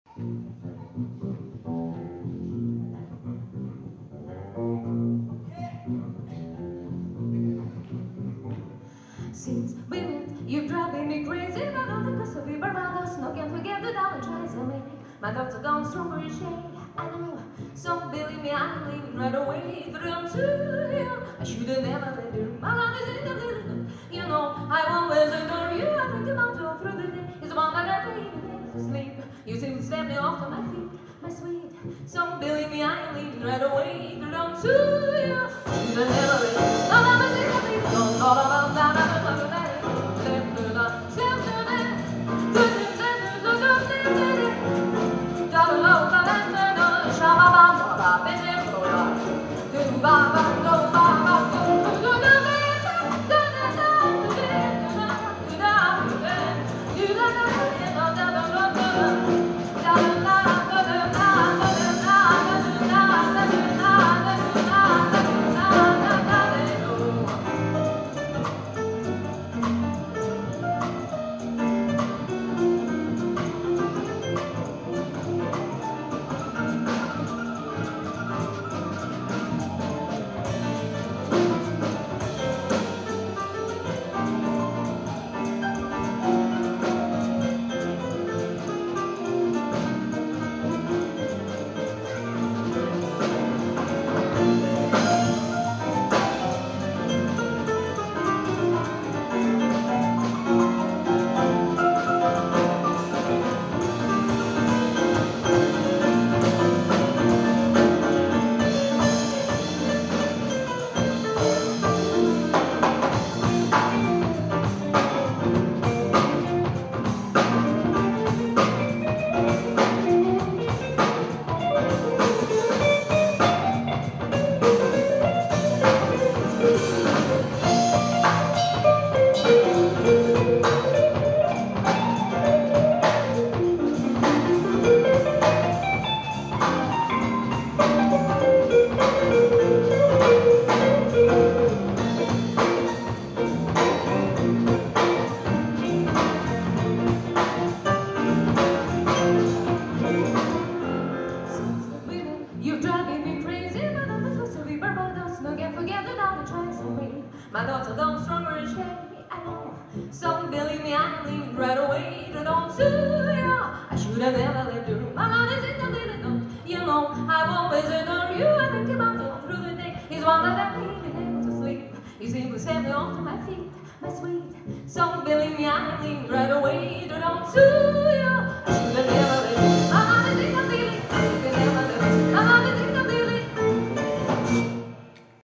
"Доктор Джаз" at "MuzEnergo 2". 2
Иркутск

Александр Глушков (гитара),
Юрий Куртилов (бас),
Наталья Лихошерст (вокал),
Алексей Тяжелухин (ударные),
Александр Филиппов (фортепиано, лидер)